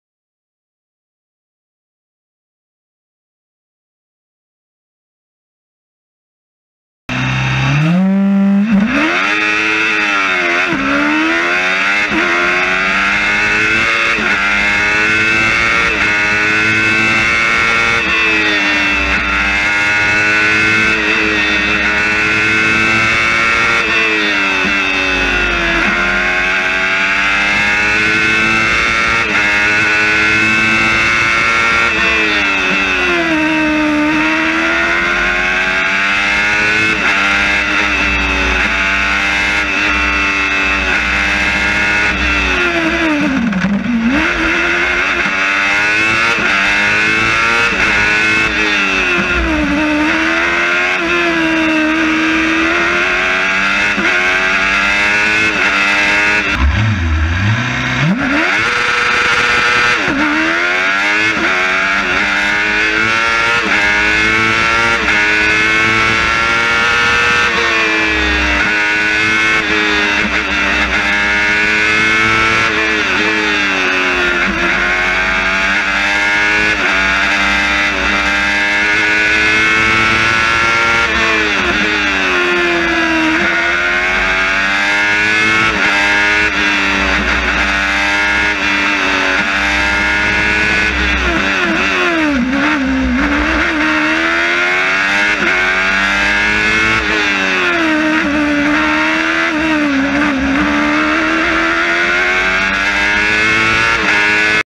Doune Hillclimb 2015 OMS Hornet
Watch Harry Pick and Steve Owen take on Doune Hillclimb in the June round of the British Hillclimb championship.